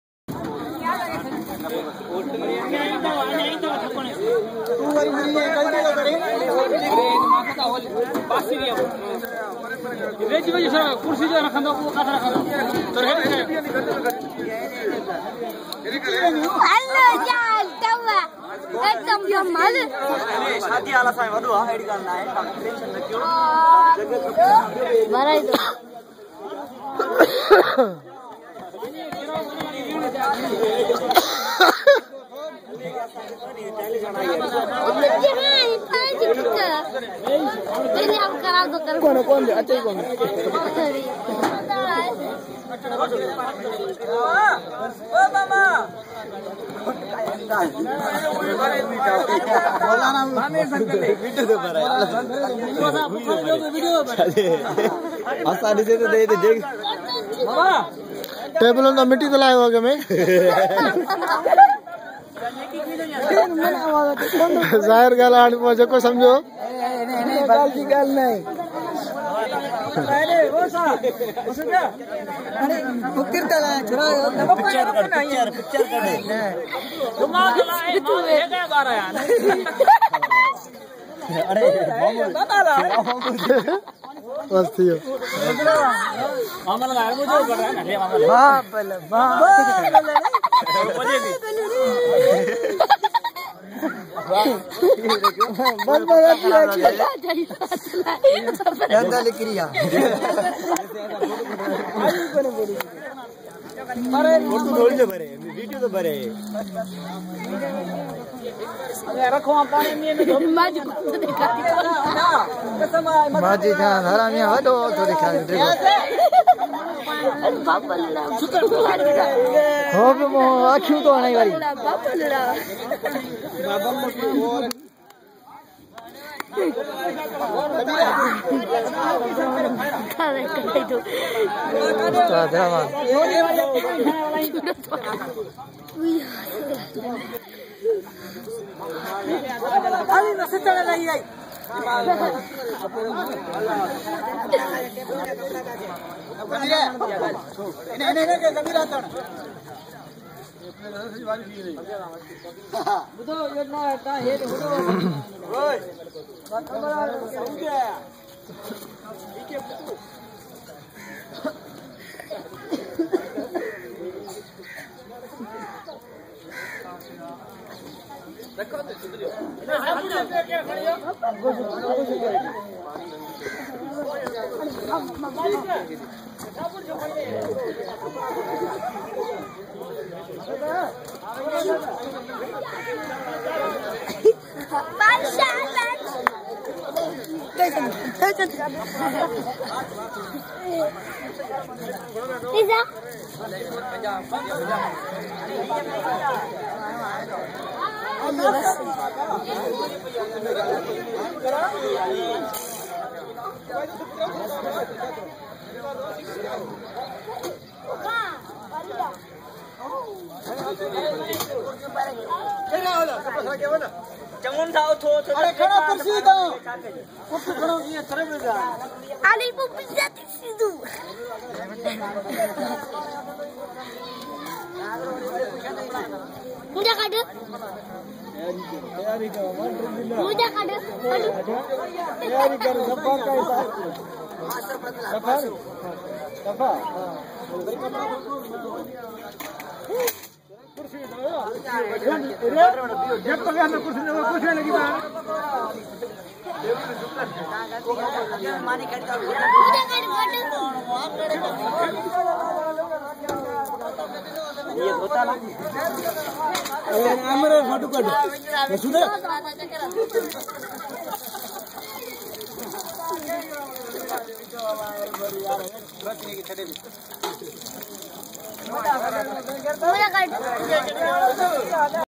SINDHI VILAGE SHADI.COM PART (2) HD
imdad aur awis mangrio ke shade k sham ko tayare men lage hoe sary apny log khoshi k aalam men .?